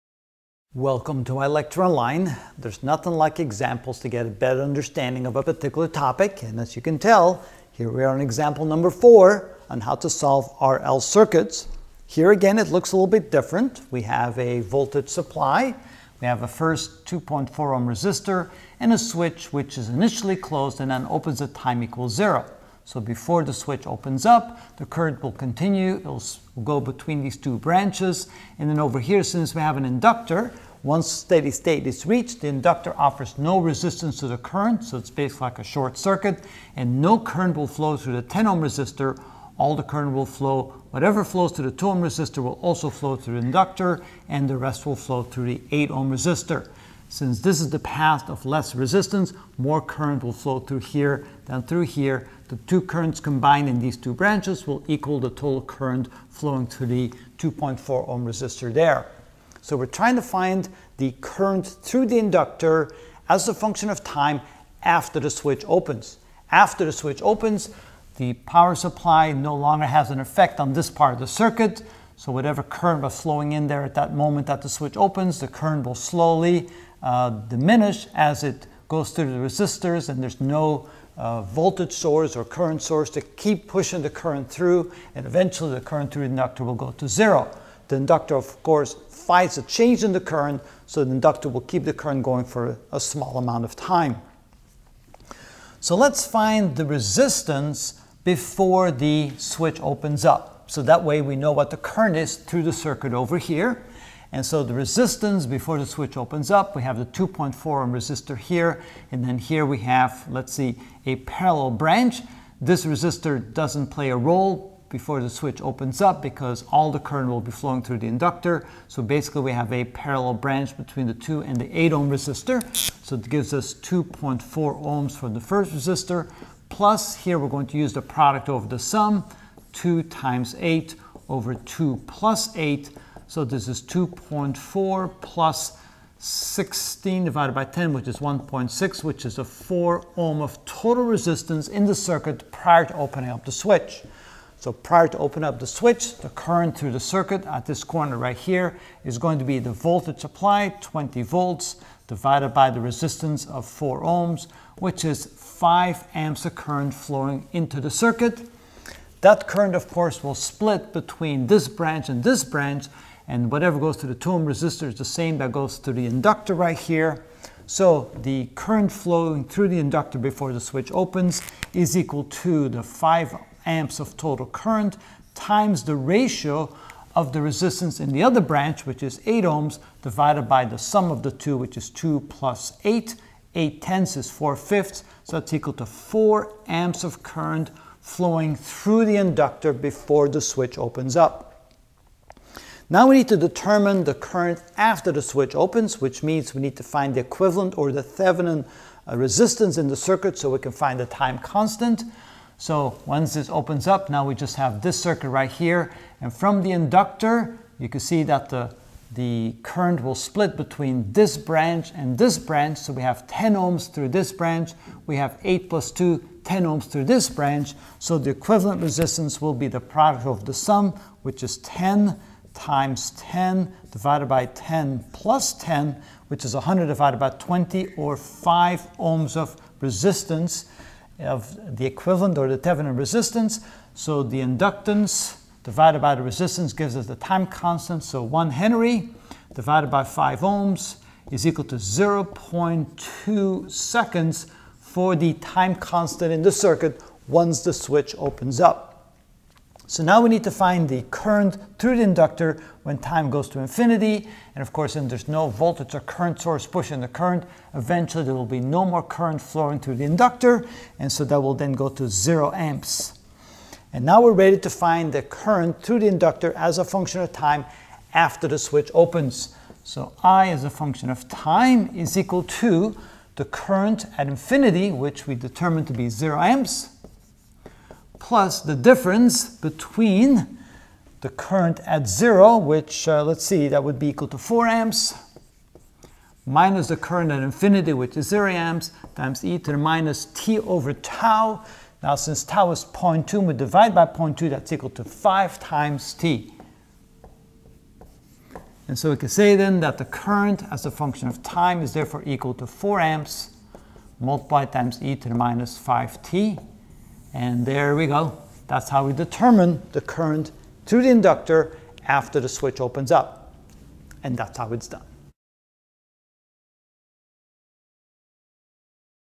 Electrical Engineering: Ch 8: RC & RL Circuits (42 of 65) General Strategy Solving RL Circuits Ex.4
In this video I will find the voltage across the capacitor(t=0)=?, voltage across the capacitor(t=infinity)=?, the time constant=?, current across the inductor=? of the RL circuits. (Example 4)